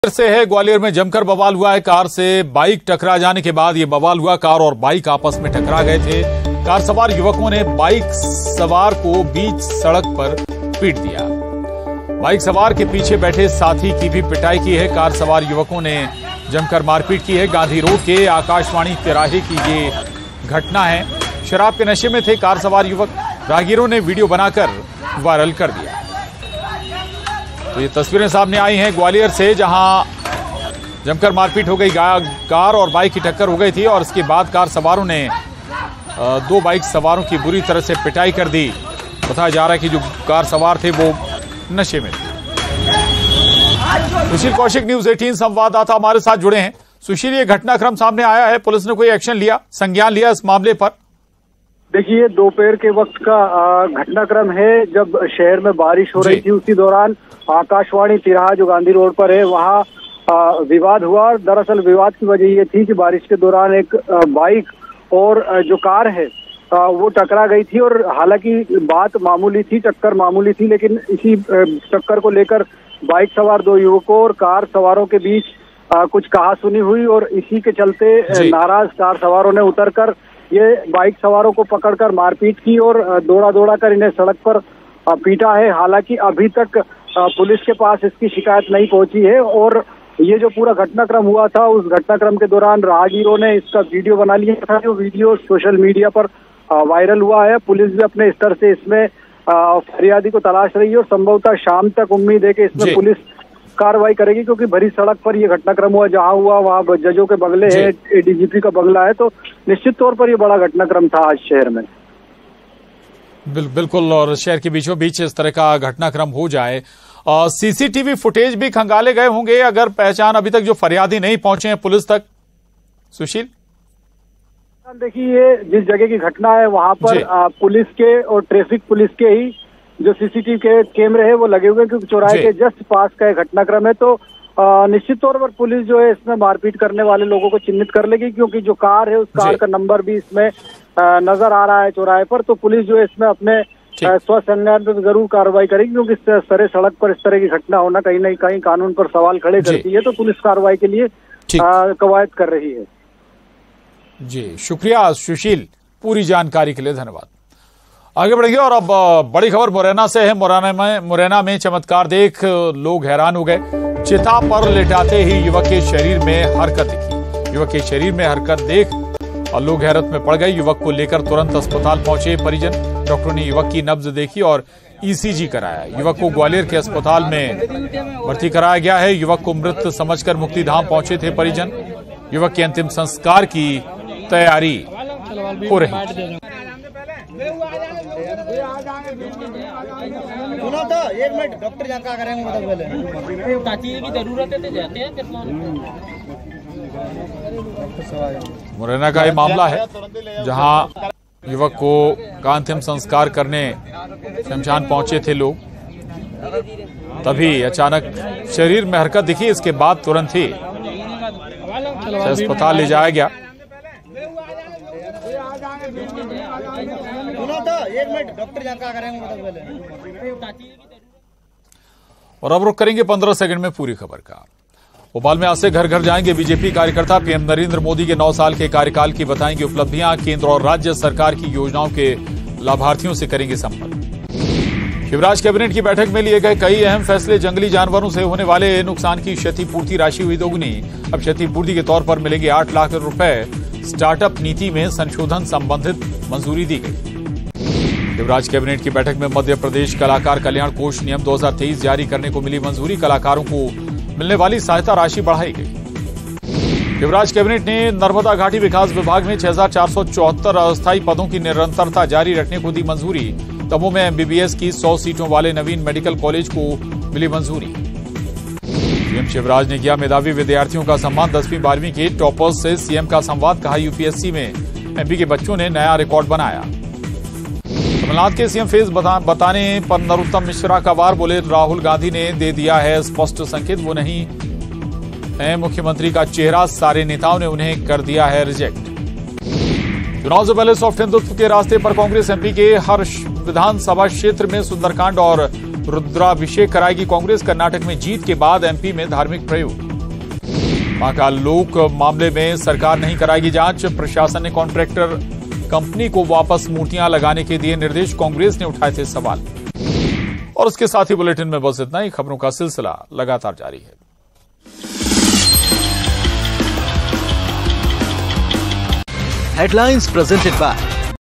Gwalior News : बीच सड़क जमकर बवाल, Car से Bike टकराने के बाद बवाल | Latest News | Breaking News
Gwalior में जमकर बवाल, कार से बाइक टकराने के बाद बवाल. कार और बाइक आपस में टकराई. कार सवार युवकों ने बाइक सवार को बीच सड़क पर पीटा. बाइक सवार के पीछे बैठे साथी की भी पिटाई की. गांधी रोड के आकाशवाणी तिराहा की घटना.